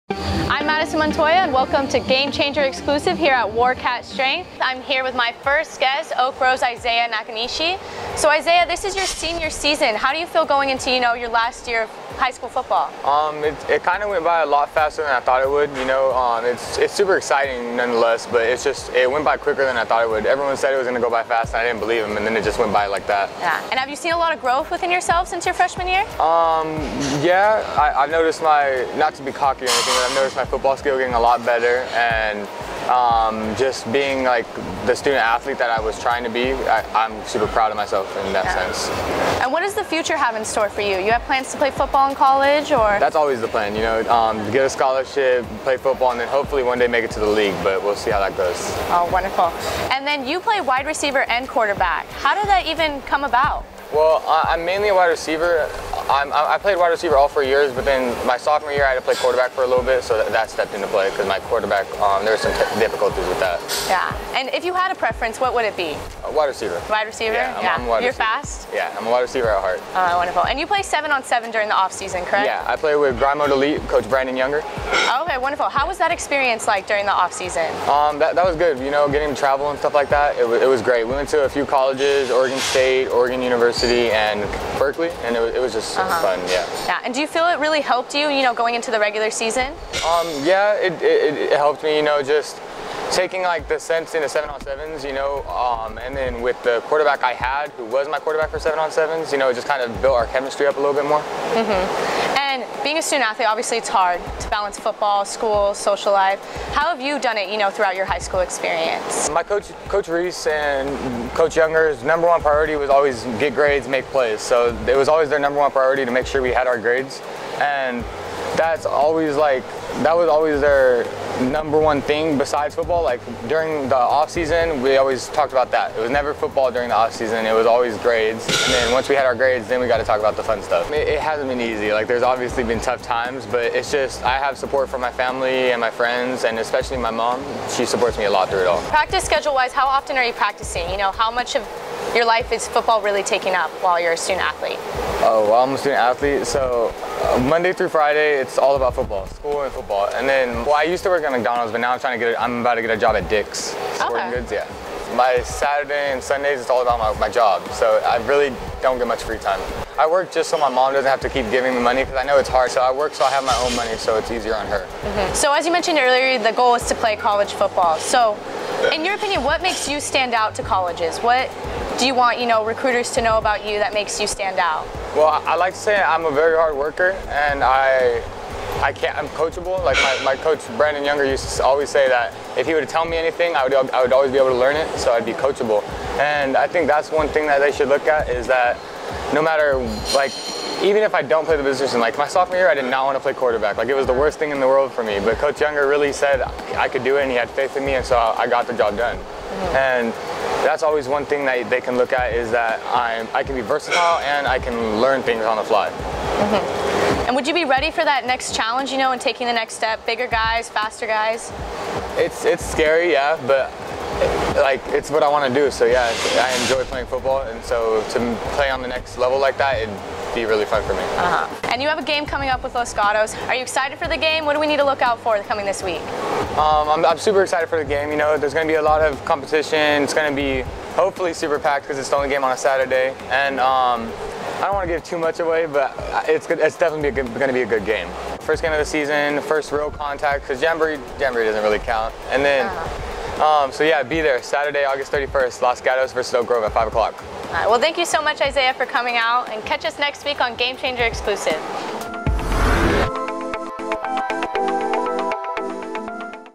Oak Grove Eagles WR/DB Izaiah Nakanishi - GetSportsFocus Game-Changer Exclusive
Oak Grove's WR, Izaiah Nakanishi, is heading into his senior season with confidence and a positive mentality. The 2018 BVAL Football All-Leaguer is a standout on and and off the field and described by his coaches as a "hard working and coachable" player.
Nakanishi is looking to lead his Eagles against the Los Gatos Wildcats this Saturday to open up the 2019 season.
Former Oak Grove football head coach and athletic director Ed Buller will be honored during pregame with a field dedication. Coach Buller won five CCS Championships in his 23 seasons as a head coach with a 214-39-3 record.